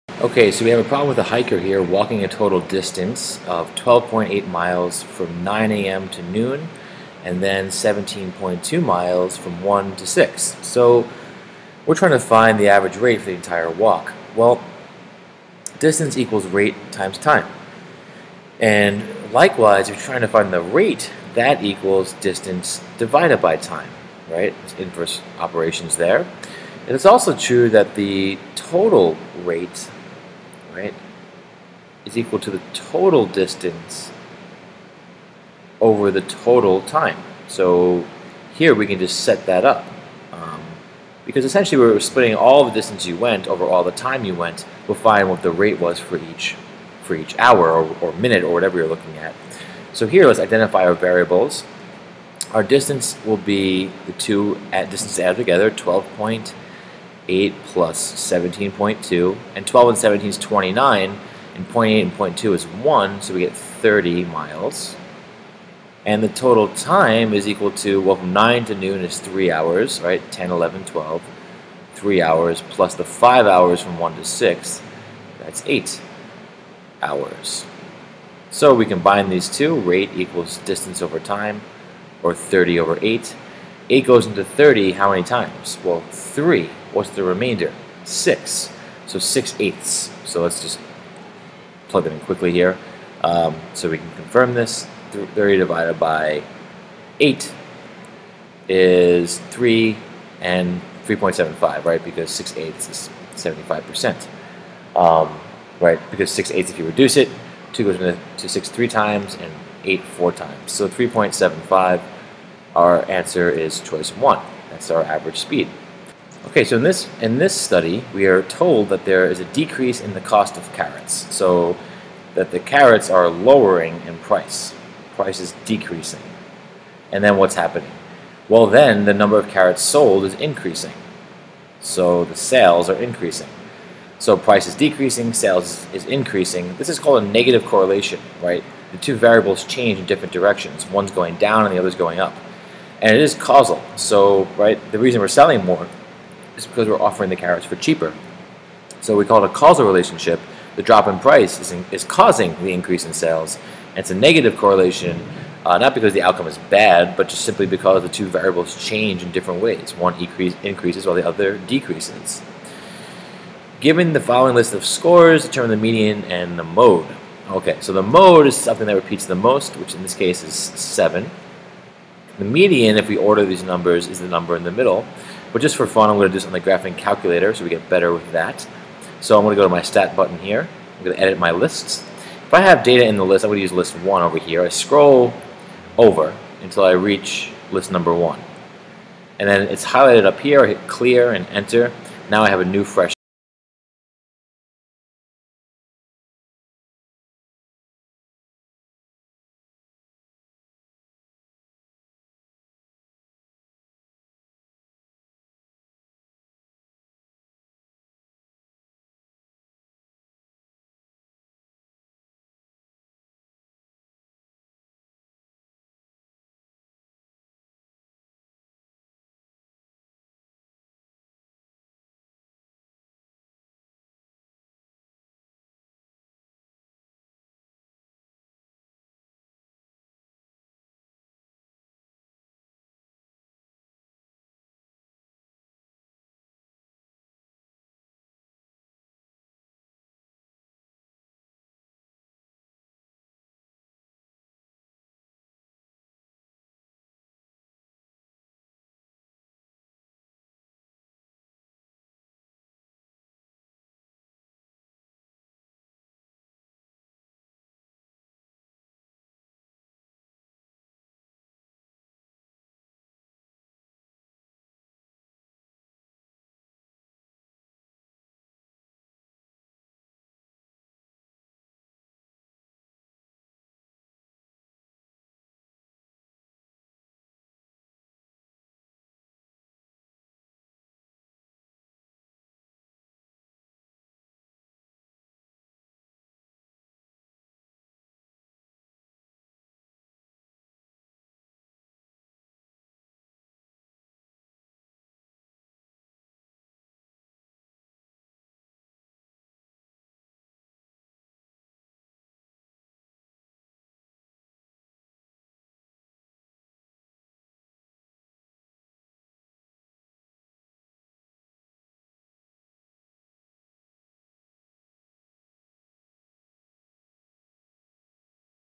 quiz 17
In this video we review a basic set of statistics problems that are used in the new york state high school regents exam